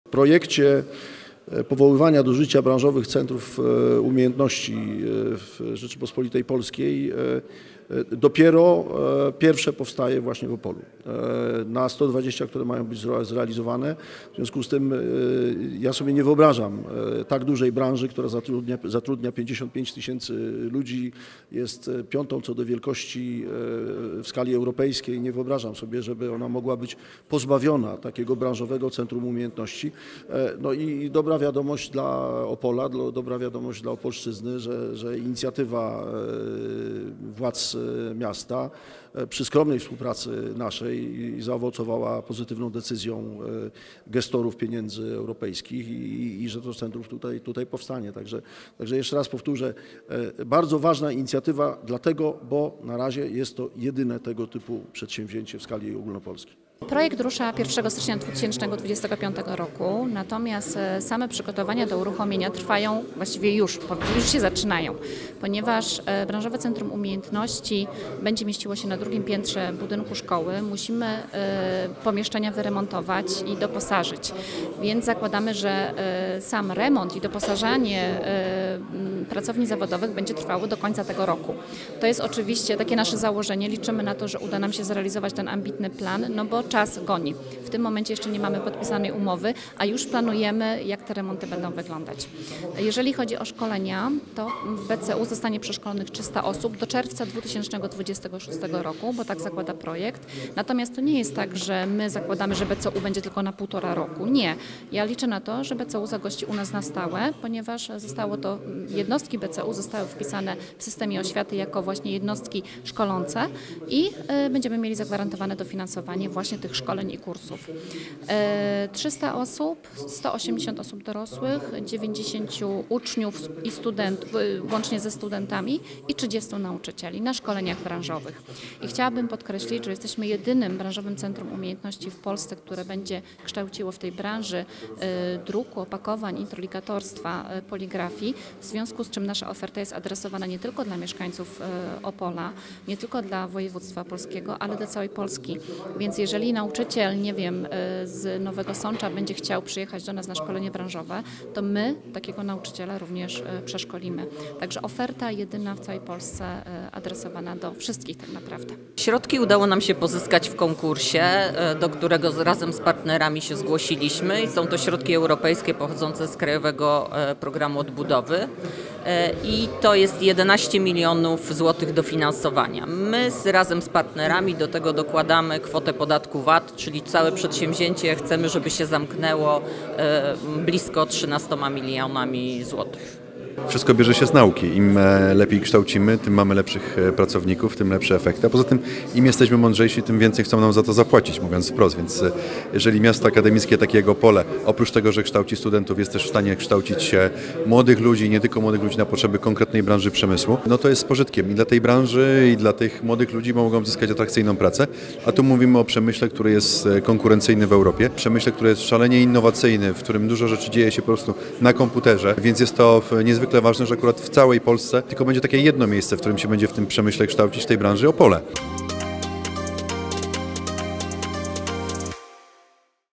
Powstaje nowe Branżowe Centrum Umiejętności
Branżowe Centrum Umiejętności w dziedzinie poligrafii, introligatorstwa i opakowań powstaje w Opolu. To będzie pierwsze i na razie jedyne takie miejsce w Polsce, dlatego też, będzie dostępne dla wszystkich zainteresowanych z całego kraju.